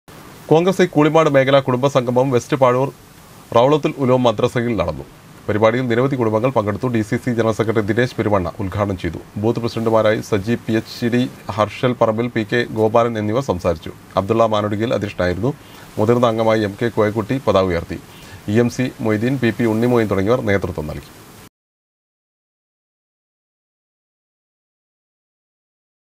കോൺഗ്രസ് ഐ കൂളിമാട് മേഖലാ കുടുംബ സംഗമം വെസ്റ്റ് പാഴൂർ റൗളത്തുൽ ഉലൂം മദ്രസയിൽ നടന്നു
കോൺഗ്രസ് ഐ കൂളിമാട് മേഖലാ കുടുംബ സംഗമം വെസ്റ്റ് പാഴൂർ റൗളത്തുൽ ഉലൂം മദ്രസയിൽ നടന്നു. പരിപാടിയിൽ നിരവധി കുടുംബങ്ങൾ പങ്കെടുത്തു